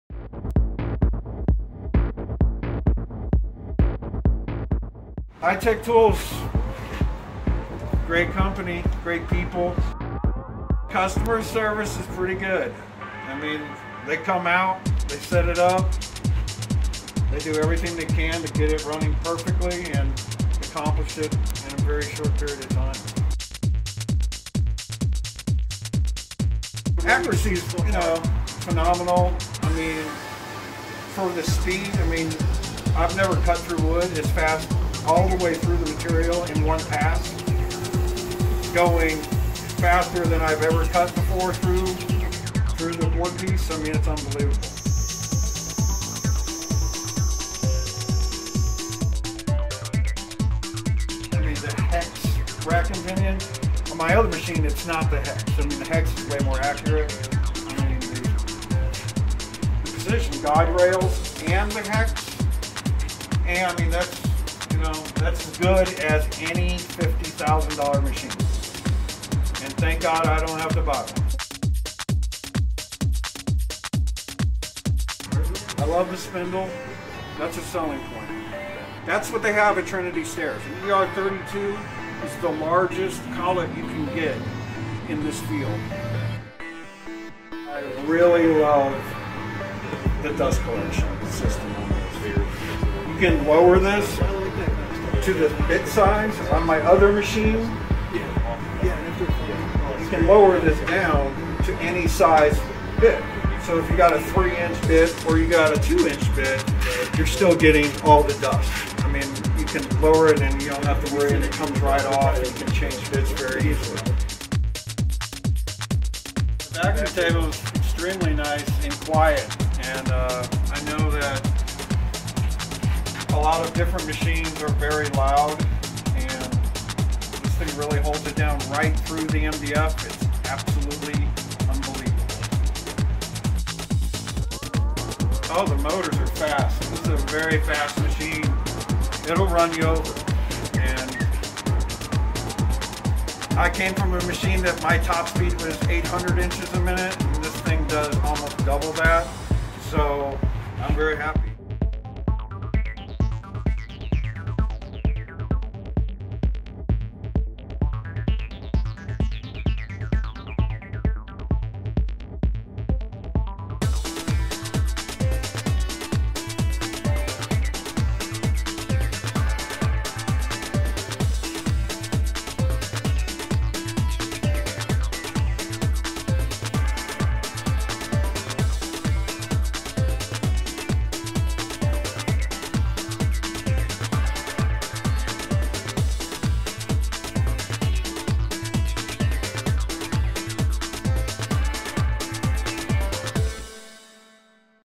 Sign Master II - HT4896 - User Review - CNC Router Table
Jeff Fanning of Dallas, Texas talks about his Sign Master II from Hytek Tools.